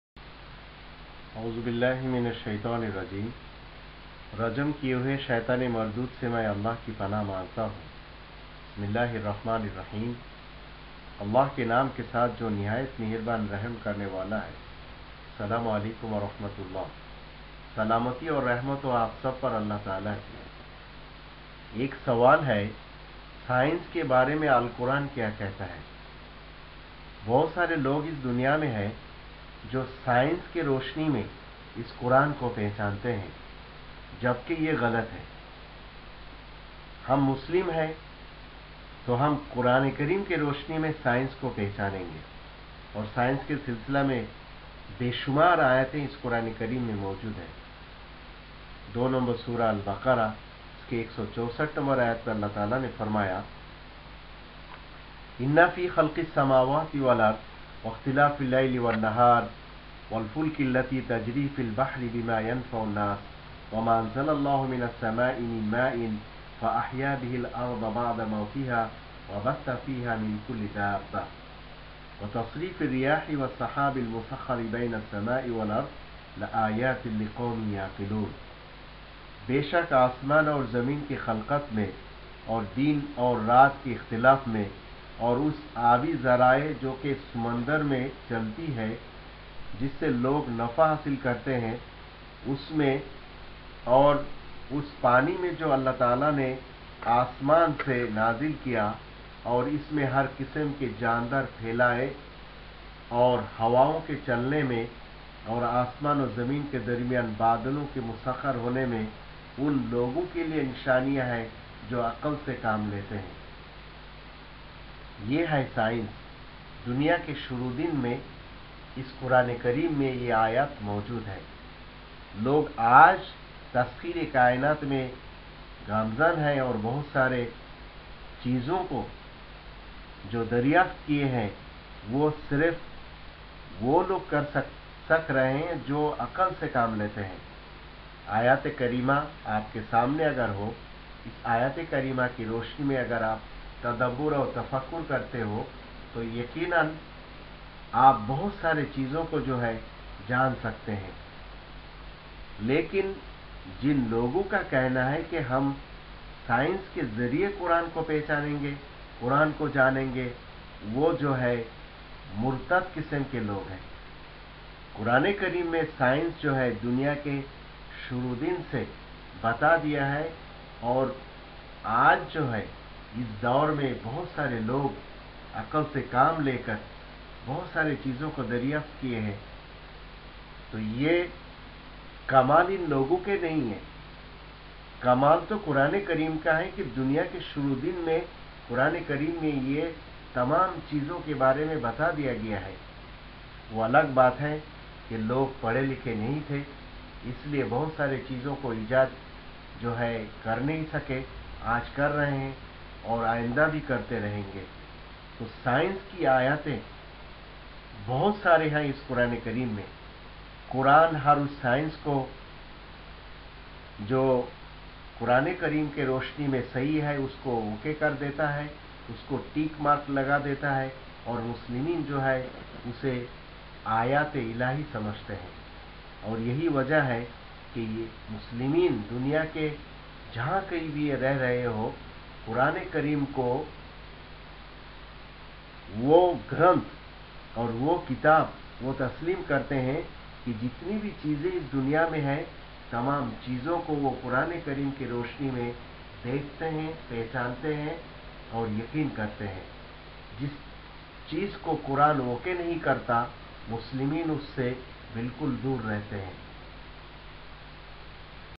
سائنس کے بارے میں قرآن کیا کہتا ہے ؟
ڈاکٹر ابوالقاسم محمّد محبوب الله جسری
حافظ، عالم، مفتی، محدث، مفسر
چیئرمین: بنگلہ دیشی شیبا پارٹی